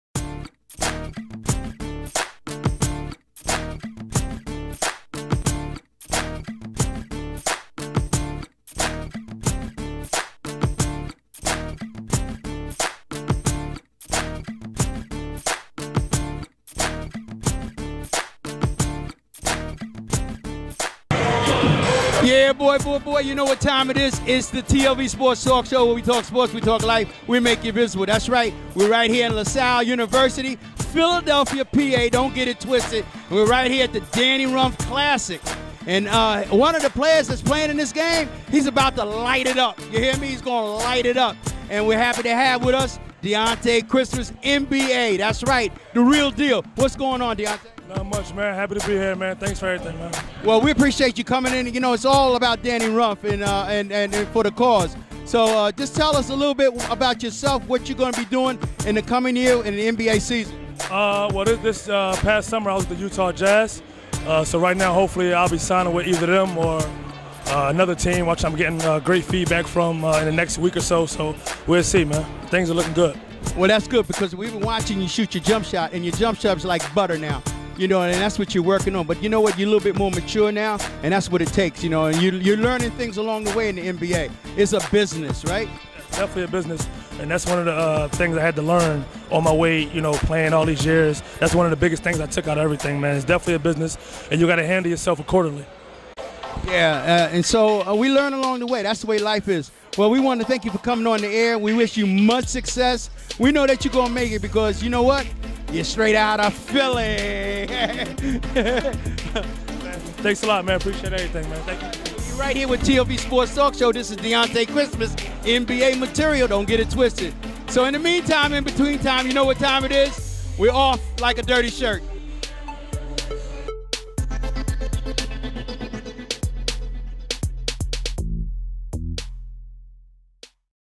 My MovieTLVSPORTSTALKSHOWDIONTECHRISTMASDANNYRUMPH2016
DANNY RUMPH CLASSIC 2016 DIONTE CHRISTMAS NBA PLAYER WITH RANDY B ON THE TLV SPORTS TALK SHOW